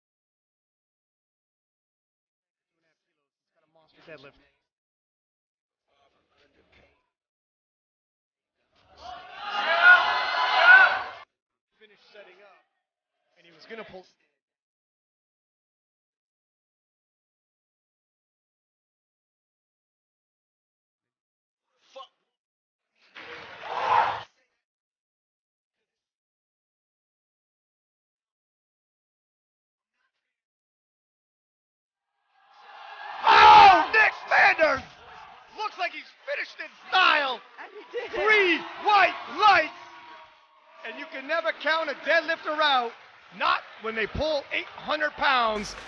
Nick Manders CAN | 3rd Place 83kg Class | 797.5kg Total | IPF Worlds 2023
Nick Manders (@n.manders) from CAN 🇨🇦 has secured his spot on the -83kg class podium! 🌍

His best lifts of the 2023 IPF World Championship in Valletta, Malta are:
Squat: 265kg
Bench press: 165kg
Deadlift: 367.5kg
For a total of 797.5kg at the bodyweight of 82.5kg

Follow: @powerlifting.wrld for more Powerlifting content! 👇